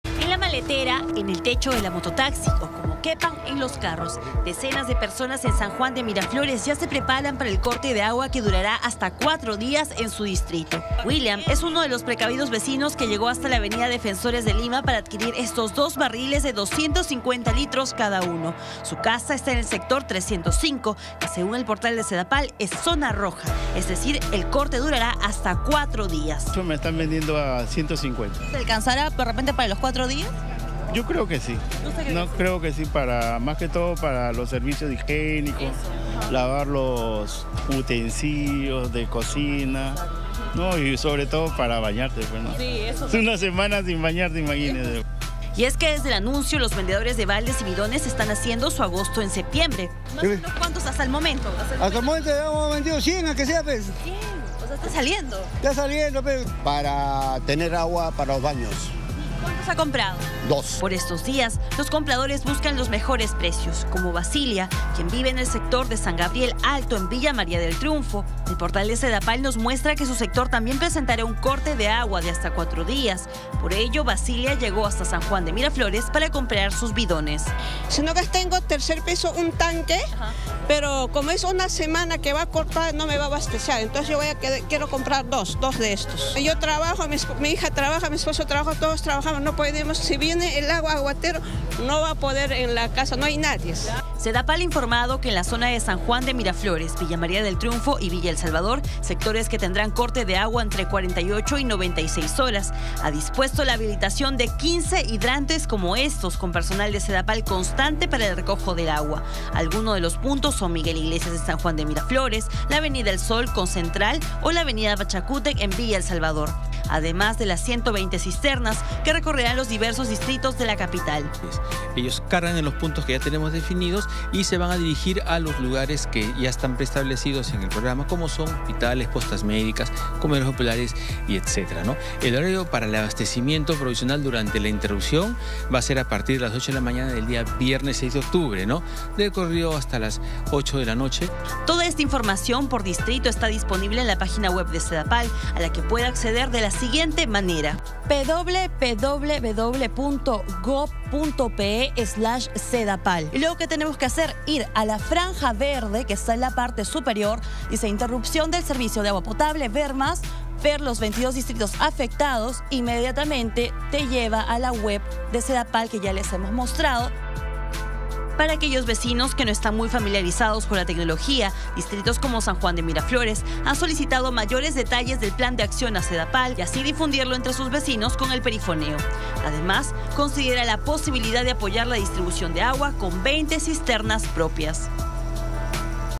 Vecinos compran cisternas y bidones por corte de agua que se extenderá por 4 días
Los vecinos de zonas que no tendrán agua por 4 días desde el 6 de octubre se alistan comprando bidones y cisternas.